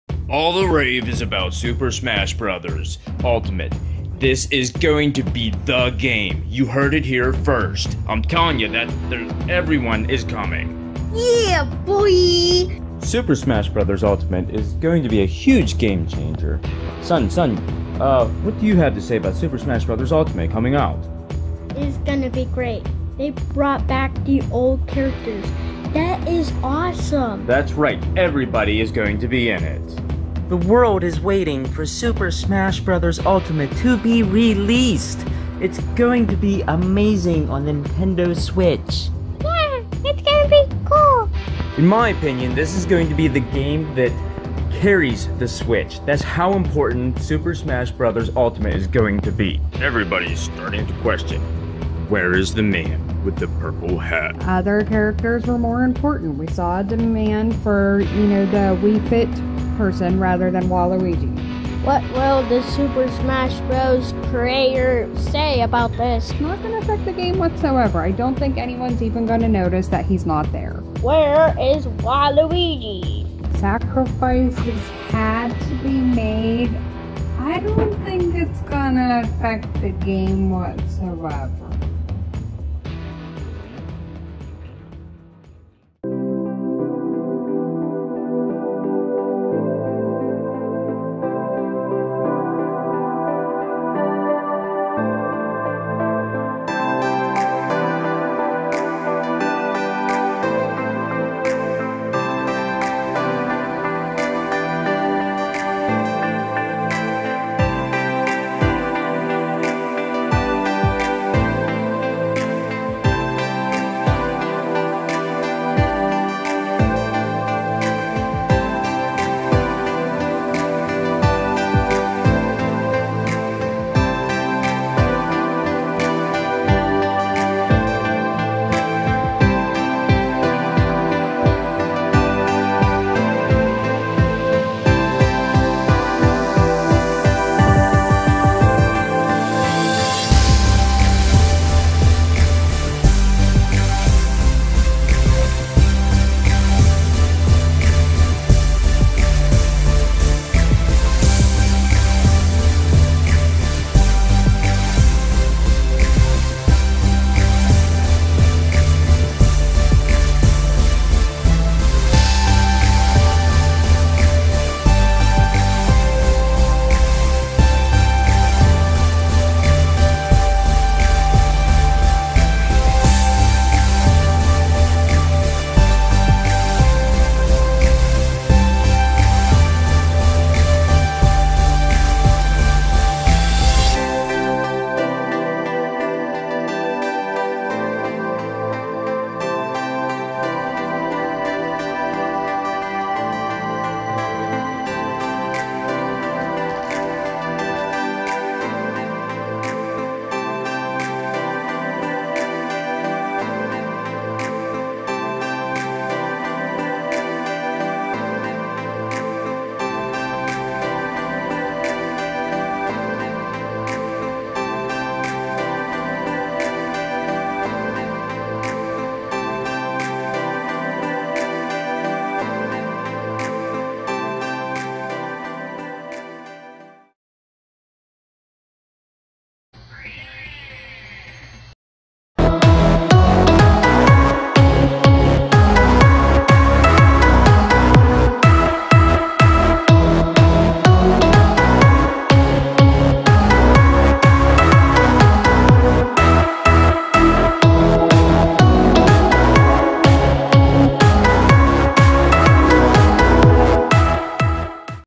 JB3 TV: Super Smash Bros. Ultimate - Waluigi, original
Our feelings on Waluigi not being in Super Smash Bros. Ultimate.